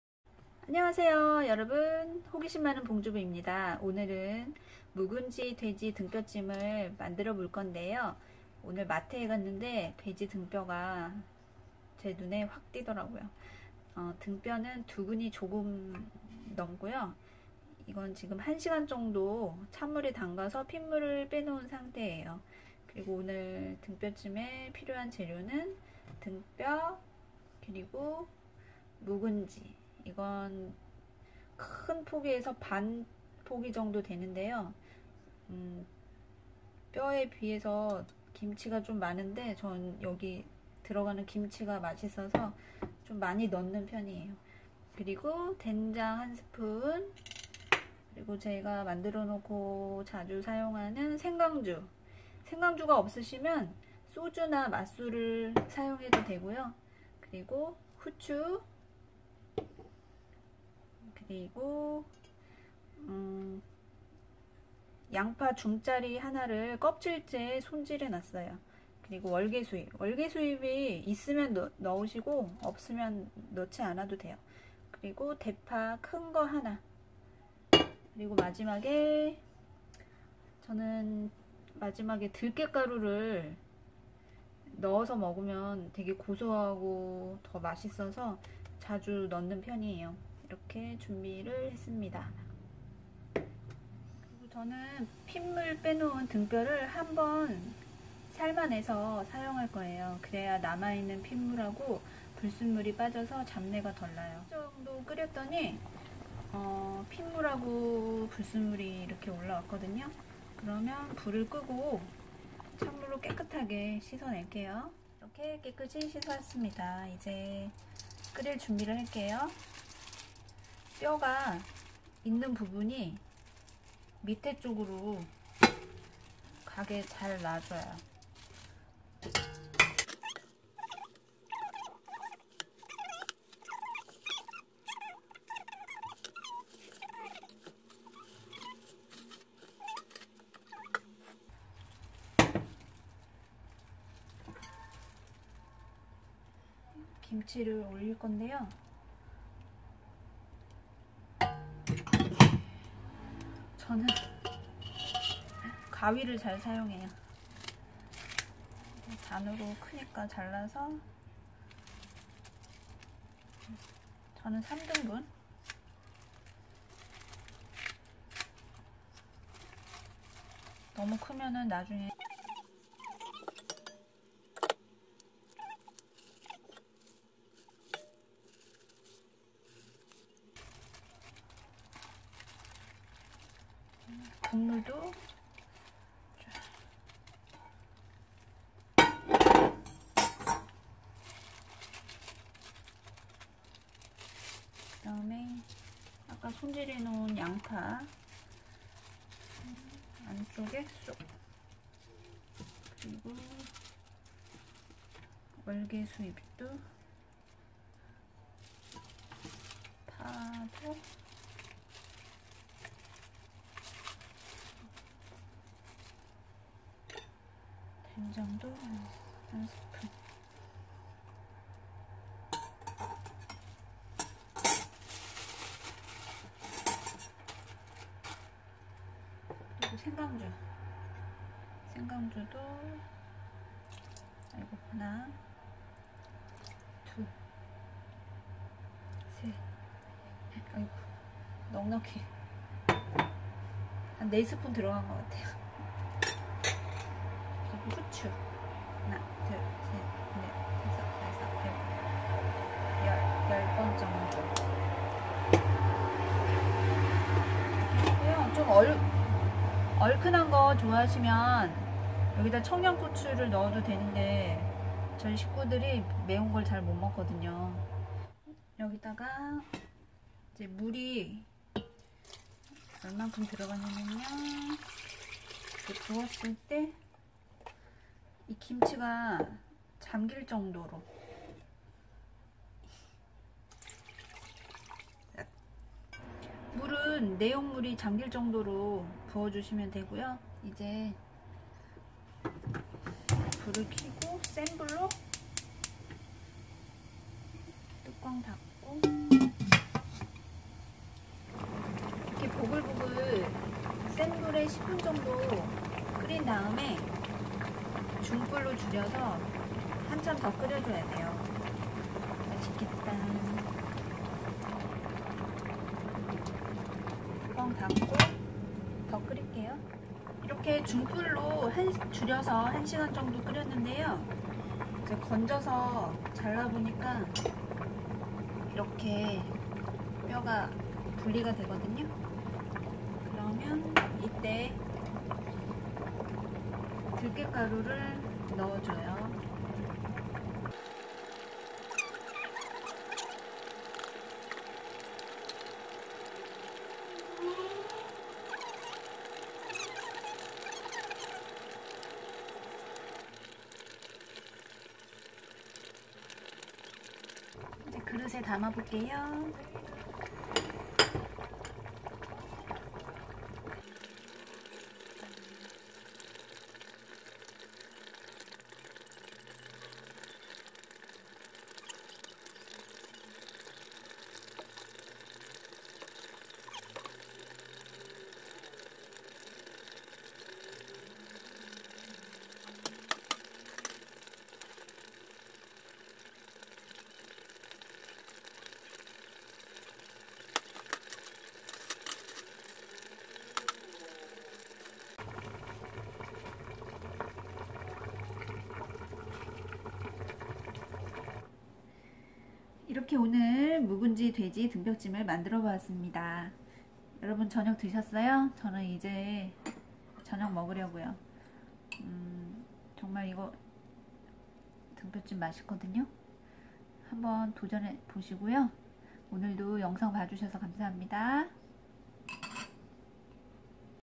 [요리] 봉주부의 국물이 구수한~ 묵은지 돼지 등뼈찜!!! [Cooking]
들깨가루가 들어가서 더욱 구수한~ 국물!